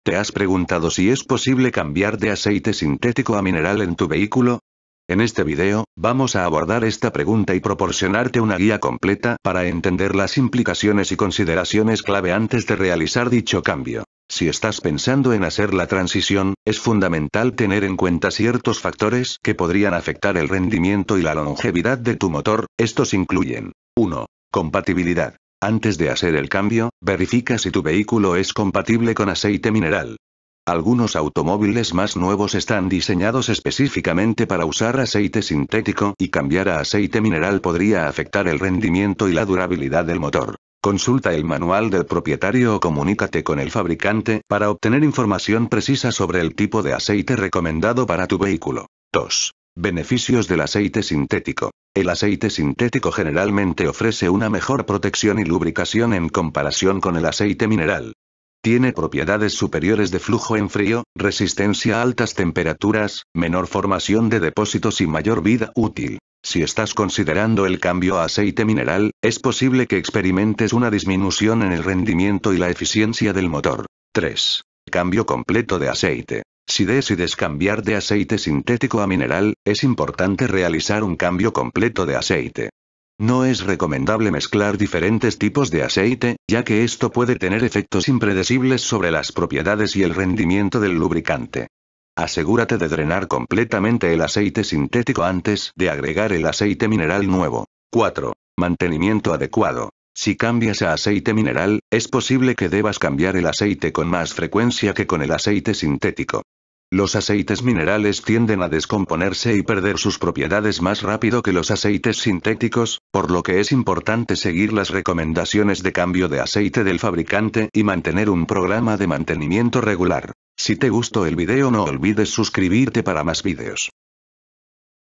💥puedo cambiar de aceite SINTETICO a MINERAL💥
¿Te has preguntado si es posible cambiar de aceite sintético a mineral en tu vehículo? En este video, vamos a abordar esta pregunta y proporcionarte una guía completa para entender las implicaciones y consideraciones clave antes de realizar dicho cambio.

 Si estás pensando en hacer la transición, es fundamental tener en cuenta ciertos factores que podrían afectar el rendimiento y la longevidad de tu motor,Estos incluyen

1

Compatibilidad:

Antes de hacer el cambio, verifica si tu vehículo es compatible con aceite mineral. Algunos automóviles más nuevos están diseñados específicamente para usar aceite sintético y cambiar a aceite mineral podría afectar el rendimiento y la durabilidad del motor.

Consulta el manual del propietario o comunícate con el fabricante para obtener información precisa sobre el tipo de aceite recomendado para tu vehículo.

2

Beneficios del aceite sintético:

El aceite sintético generalmente ofrece una mejor protección y lubricación en comparación con el aceite mineral. Tiene propiedades superiores de flujo en frío, resistencia a altas temperaturas, menor formación de depósitos y mayor vida útil.

Si estás considerando el cambio a aceite mineral, es posible que experimentes una disminución en el rendimiento y la eficiencia del motor.

3

Cambio completo de aceite:

 Si decides cambiar de aceite sintético a mineral, es importante realizar un cambio completo de aceite. No es recomendable mezclar diferentes tipos de aceite, ya que esto puede tener efectos impredecibles sobre las propiedades y el rendimiento del lubricante. Asegúrate de drenar completamente el aceite sintético antes de agregar el aceite mineral nuevo.

4

Mantenimiento adecuado:

Si cambias a aceite mineral, es posible que debas cambiar el aceite con más frecuencia que con el aceite sintético. Los aceites minerales tienden a descomponerse y perder sus propiedades más rápido que los aceites sintéticos, por lo que es importante seguir las recomendaciones de cambio de aceite del fabricante y mantener un programa de mantenimiento regular.